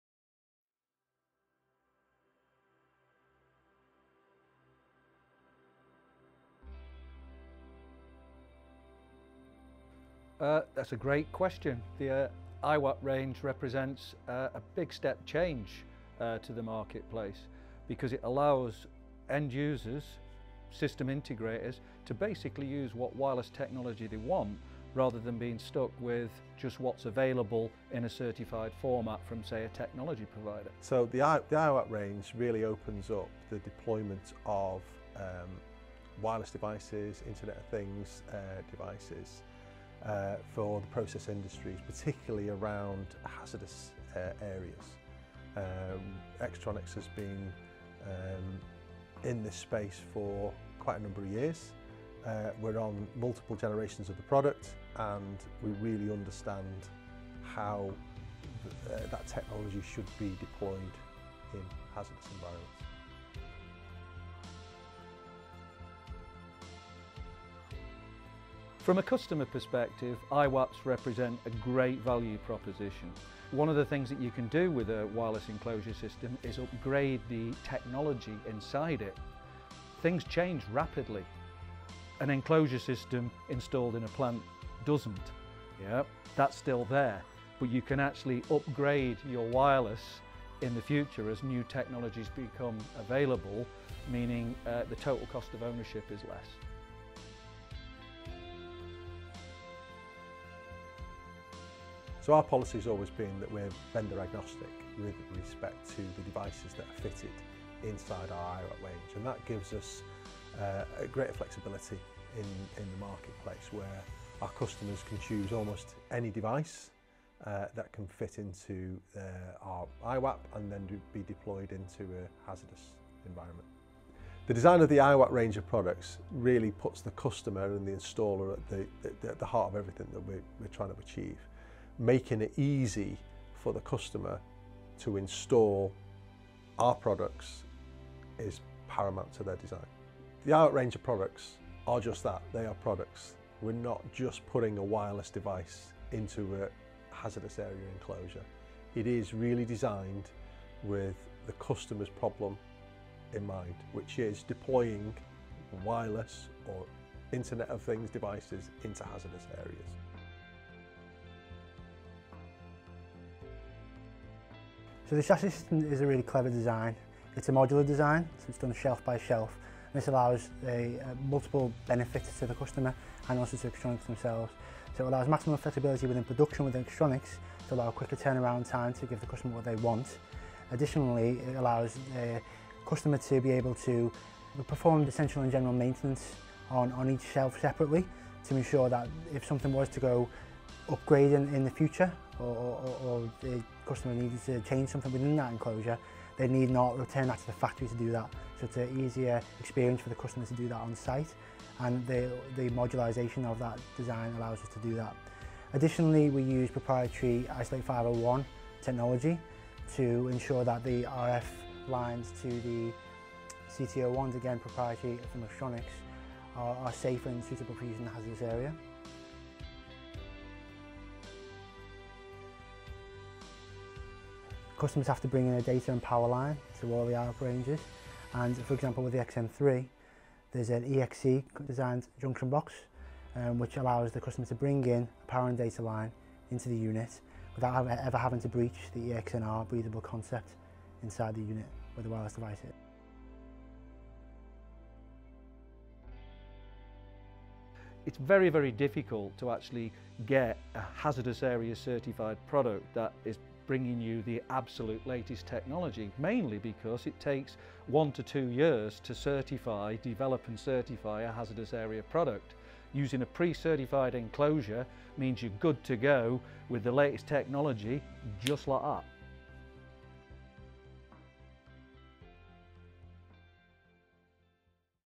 Wireless Solution Overview - iWAP Hazardous Area Wireless Enclosures
This video provides an overview of Extronics iWAP range of hazardous area wireless encloosures for the process industries.